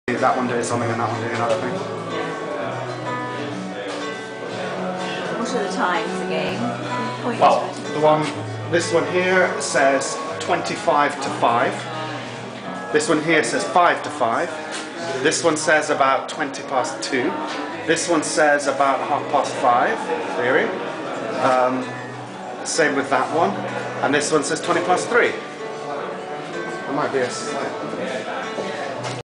Curious clocks
A novel new clock design by TOBY House - as seen at Clerkenwell Design Week 2012